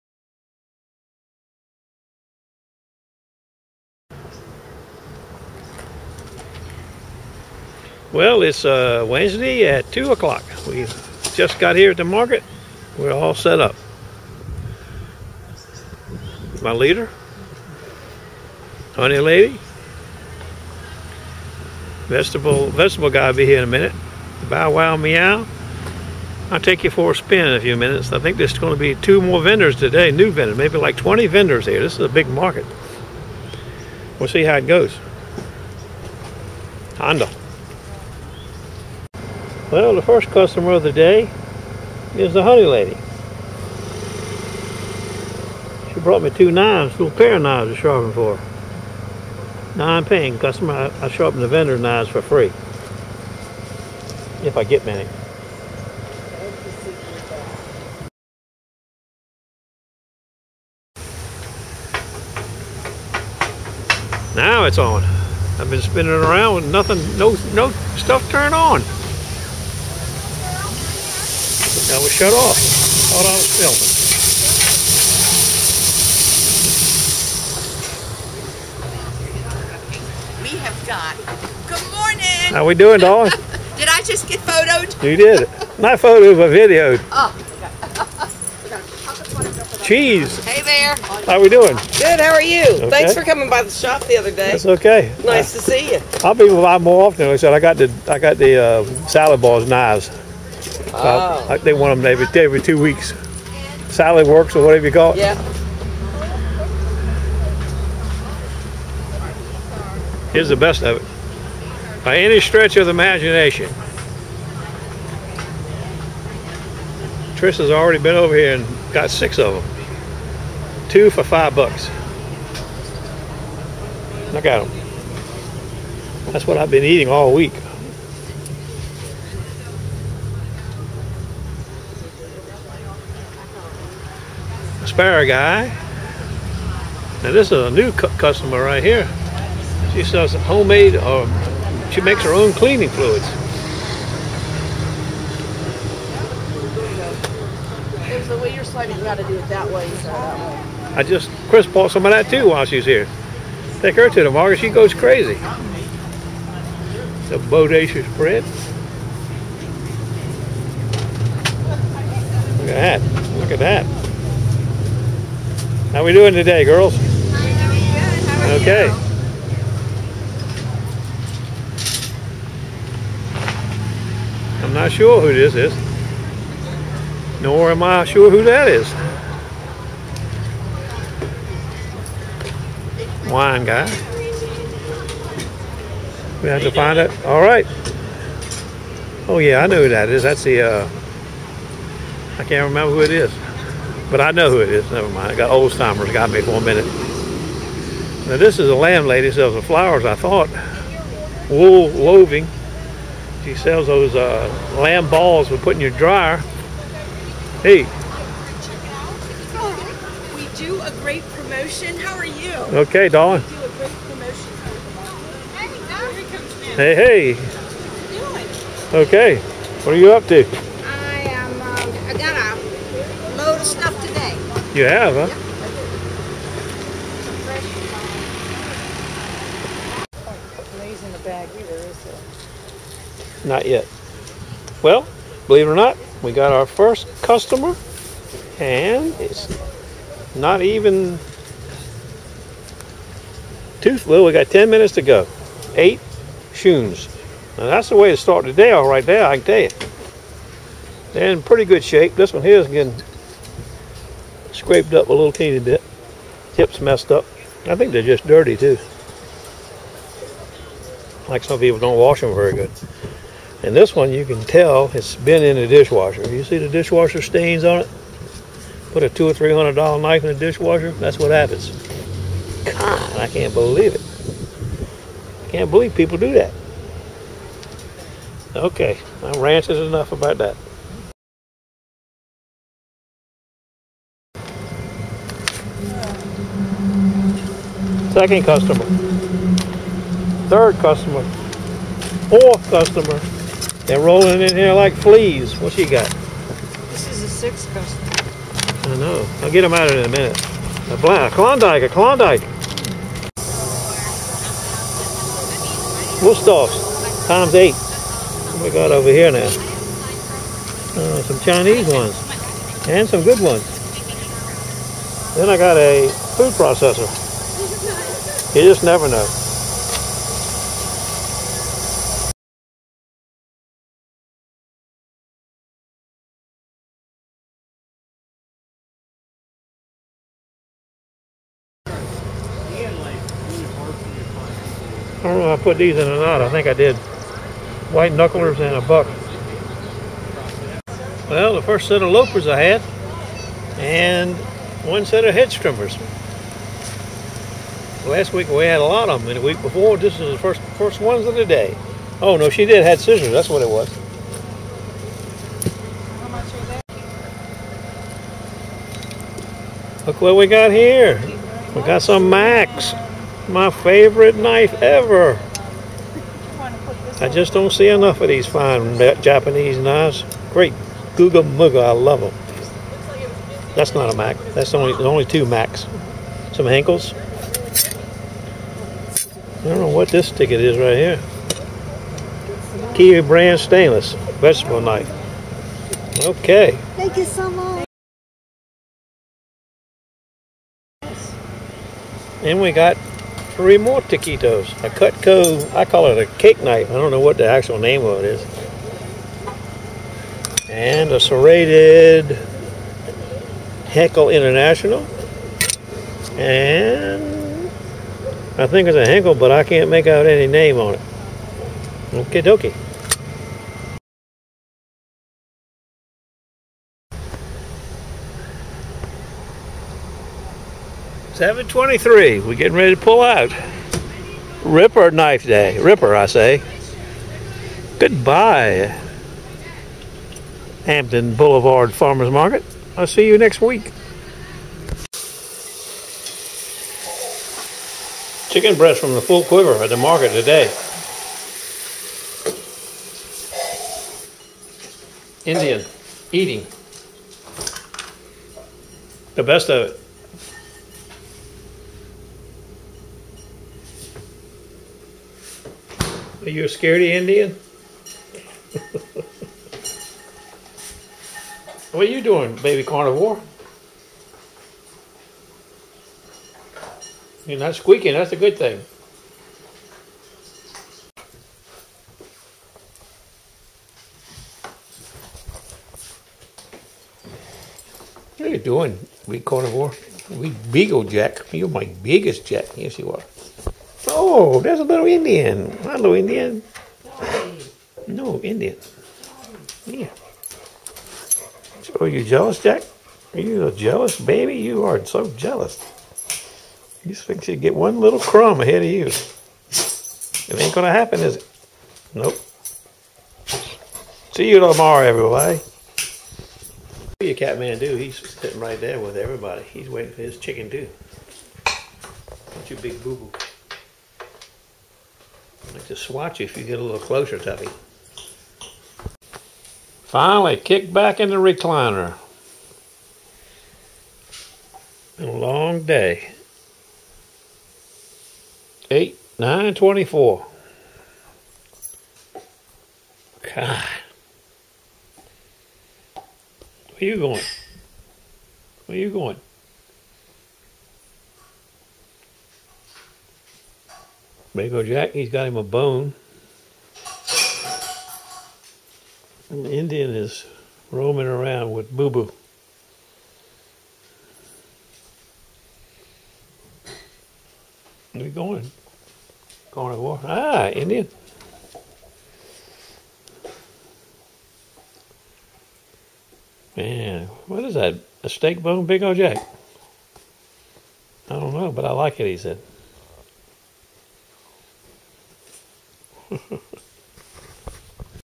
Hampton Blvd Farmers Mkt. today - Ripper Knife Day = 70 Blades
this market is turning into one of the best i go too. it was kinda like this back in 2013-14 also. just never as good as of late. i hope it continues.

see you tomorrow for the first thursday market at kings grant baptist church - 4 to 7pm. every other thursday.

GEAR I USE:
Stavrscoop
go pro hero session camera